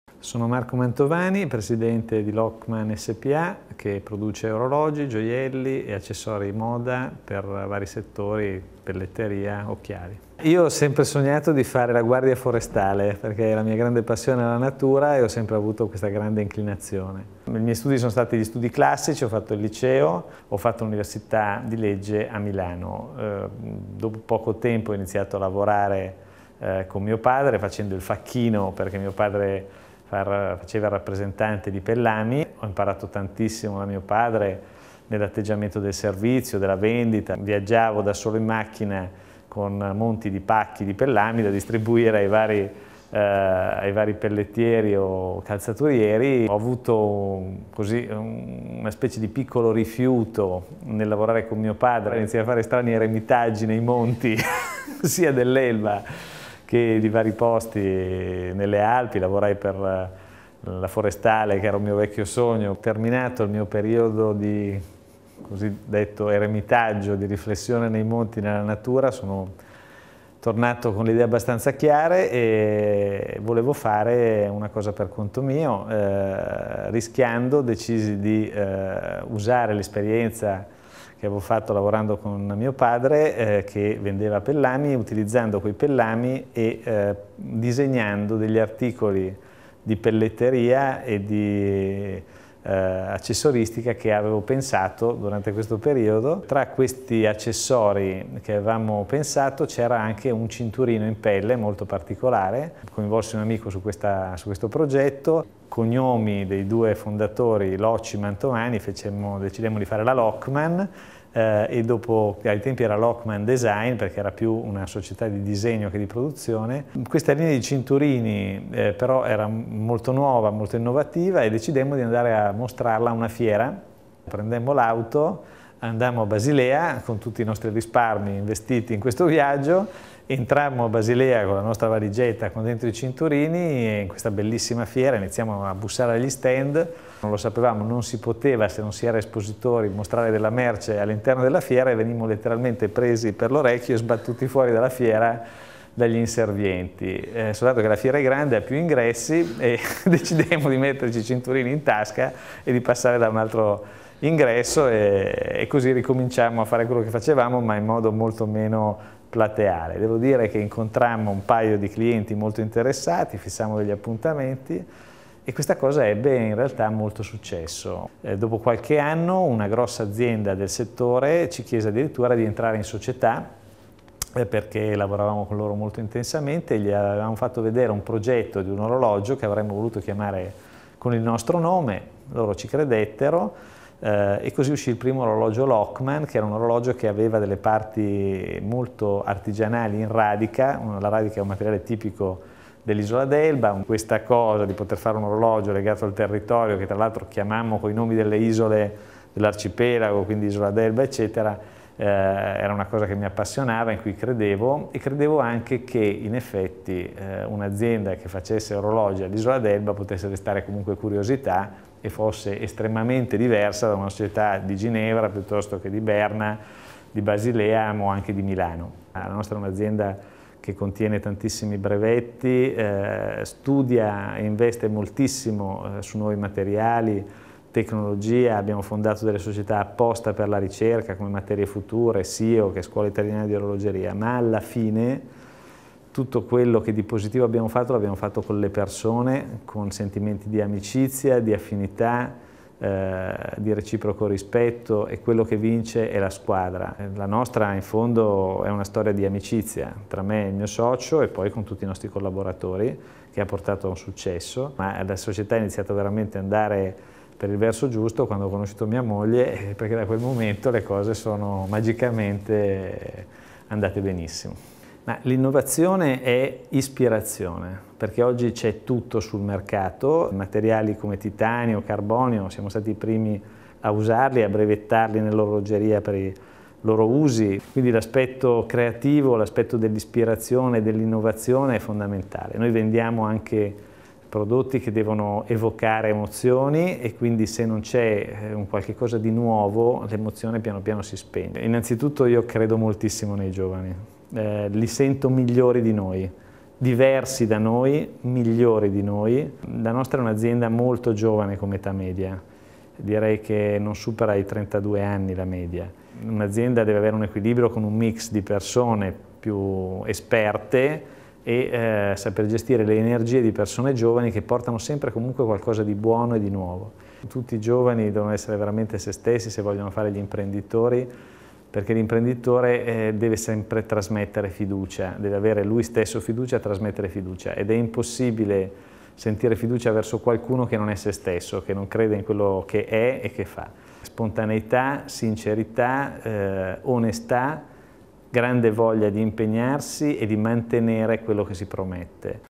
Marco Mantovani
Marco Mantovani (Locman S.p.A., LI) racconta la sua storia di imprenditore nell'ambito del progetto "La tua idea di impresa".